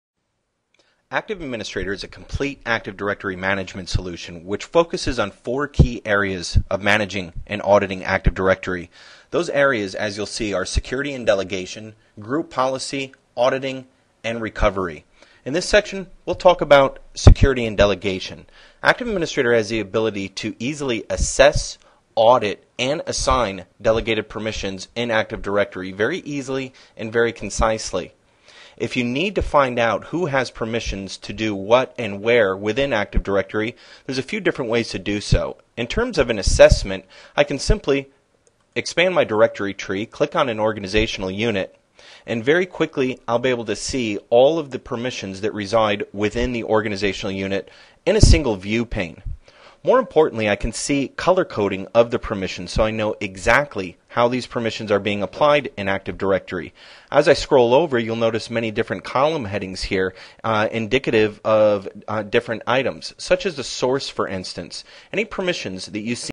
Active Directory Security Management and Delegation with Active Administrator
Active Directory security, Group Policy Management, AD auditing and reporting, featuring offline GPO repository and AD object-level backup/restore